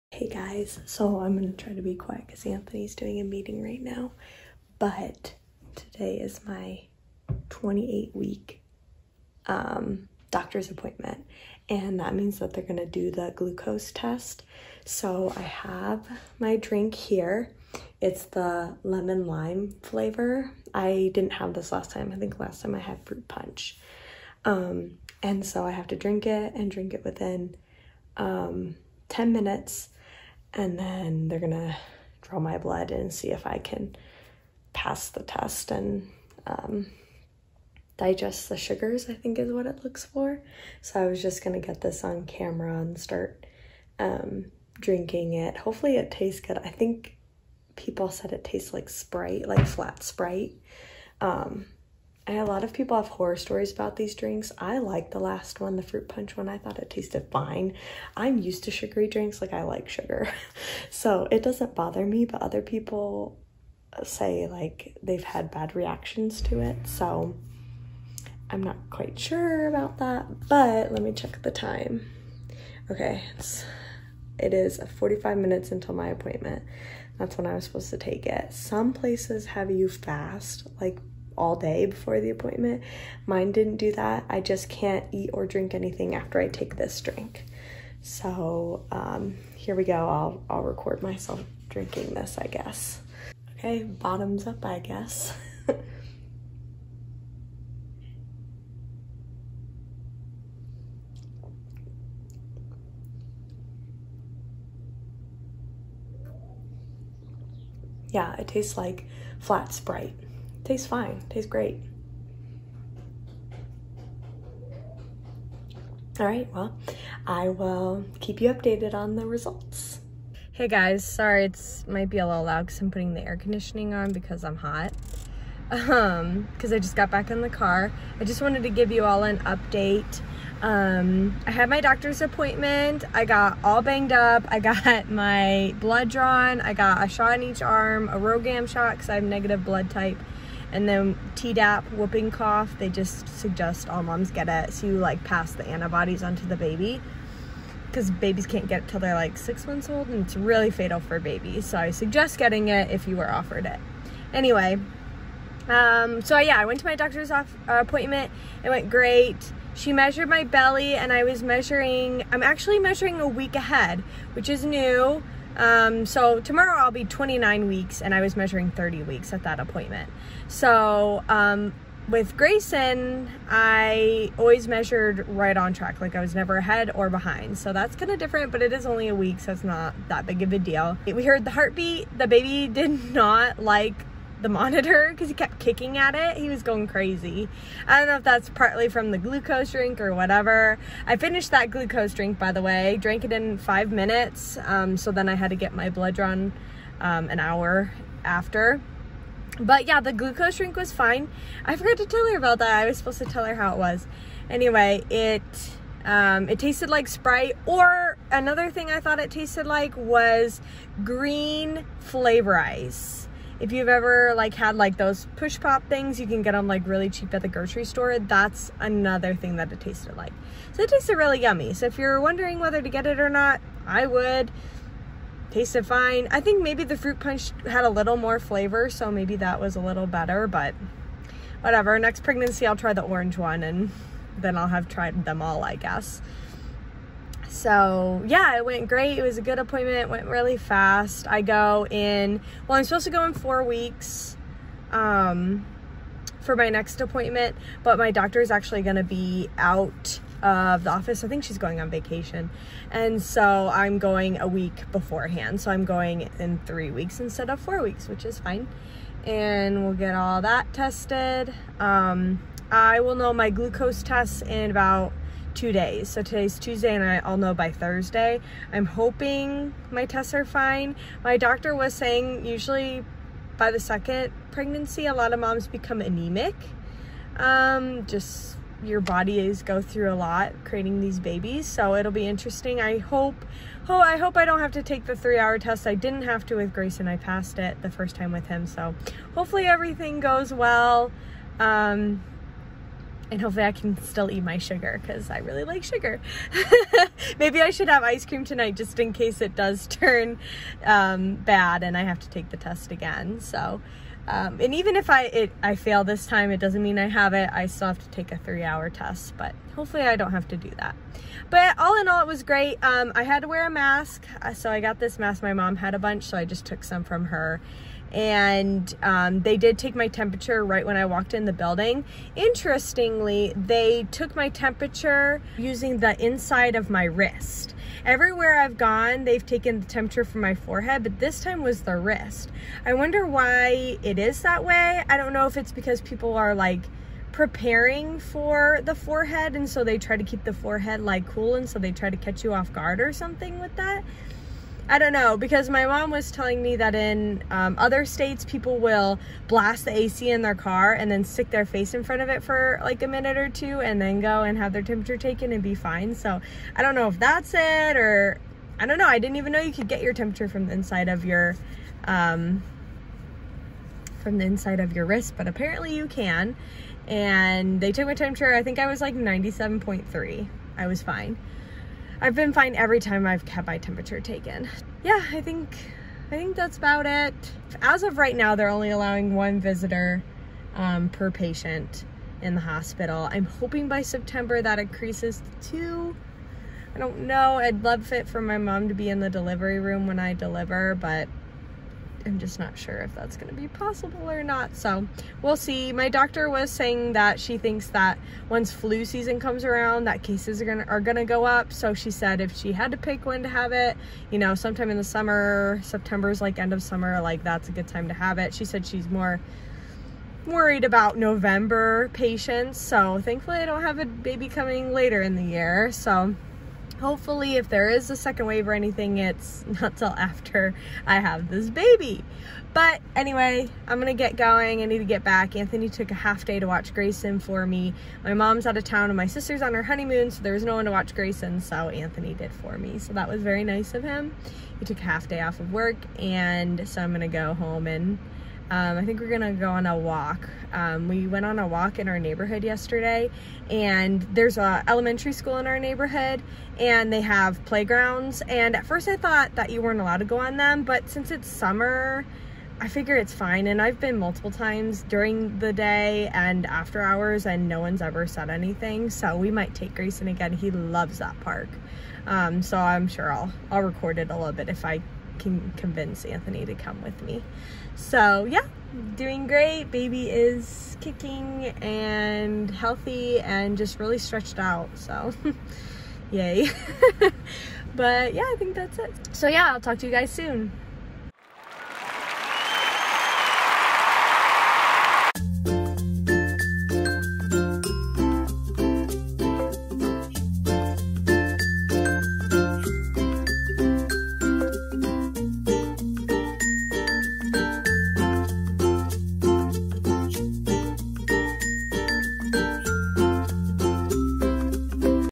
28 Week Doctors Appointment/Glucose Test!
Shannon goes to her 28 week doctors appointment and tells you how it went!